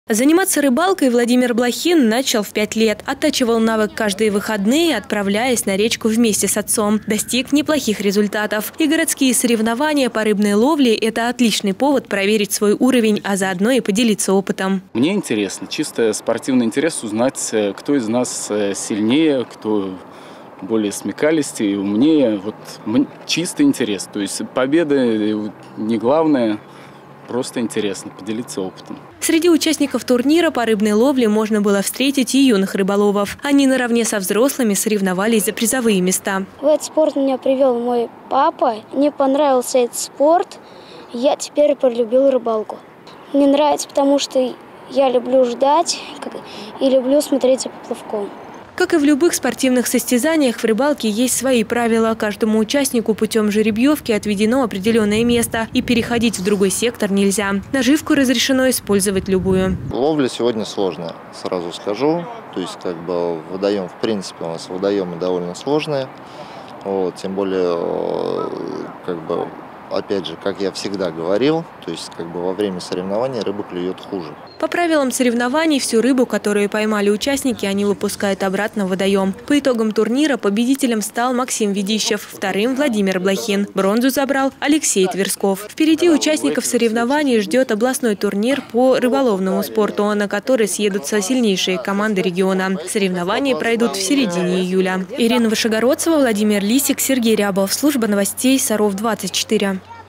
Городские соревнования по рыбной ловле
Стали известны результаты Чемпионата и Первенства города по ловле на поплавочную удочку. Организатором традиционных соревнований выступила Федерация Рыболовного Спорта.